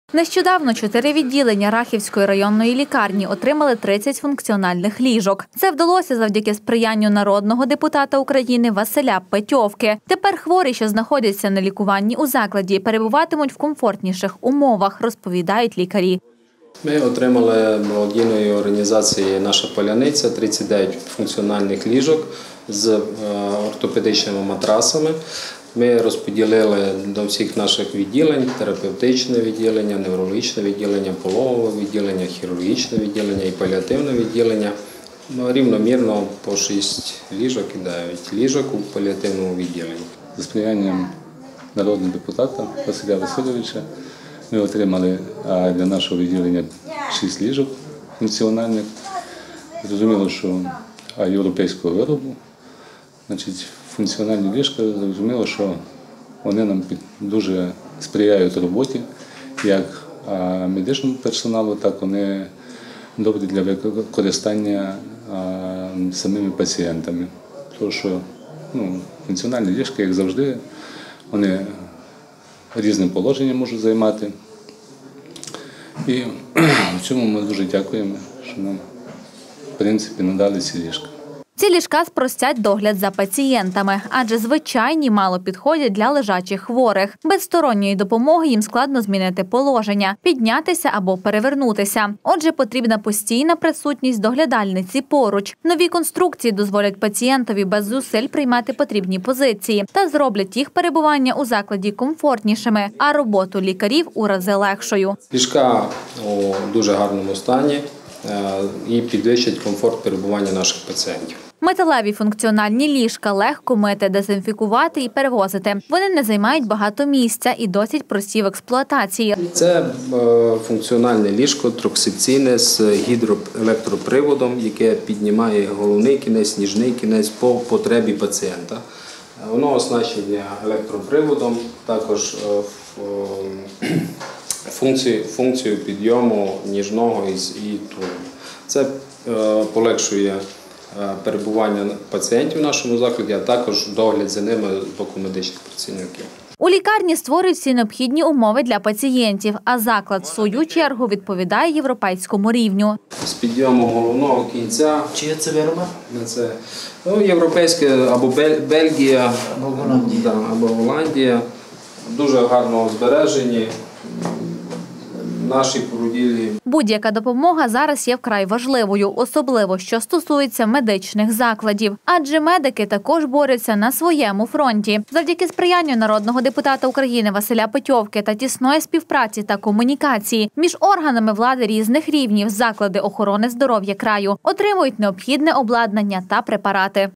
Отримали функціональні ліжка. Підвищують комфорт
До Рахівської районної лікарні за сприяння народного депутата України Василя Петьовки надійшли 30 функціональних ліжок. Нове обладнання, яке вдалося отримати допоможе у роботі медичного персоналу та сприятиме комфортнішому перебуванню пацієнтам. Нещодавно 4 відділення Рахівської районної лікарні отримали 30 функціональних ліжок. Це вдалося завдяки сприянню народного депутата України Василя Петьовки. Тепер хворі, що знаходяться на лікуванні у закладі перебуватимуть у комфортніших умовах, розповідають лікарі.  Ці ліжка спростять догляд за пацієнтами,  адже звичайні  мало підходять для лежачих хворих: без сторонньої допомоги їм складно змінити положення, піднятися або перевернутися. Отже, потрібна постійна присутність доглядальниці поруч. Нові конструкції дозволяють  пацієнтові без зусиль приймати потрібні позиції та зроблять їх перебування у закладі комфортнішими, а роботу лікарів у рази легшою. Металеві функціональні ліжка  легко мити, дезінфікувати і перевозити, вони не займають багато місця і досить прості в експлуатації, розповідає директор  установи.  У лікарні створюють всі необхідні умови для пацієнтів, а лікарня в свою чергу відповідала європейському рівню.  Допомога на всіх рівнях зараз є вкрай важливою. Особливо, що стосується медичних закладів. Адже медики також борються на своєму фронті. Завдяки сприянню народного депутата України Василя Петьовки та тісної співпраці та комунікації між органами влади різних рівнів заклади охорони здоров’я краю отримують необхідне обладнання та препарати.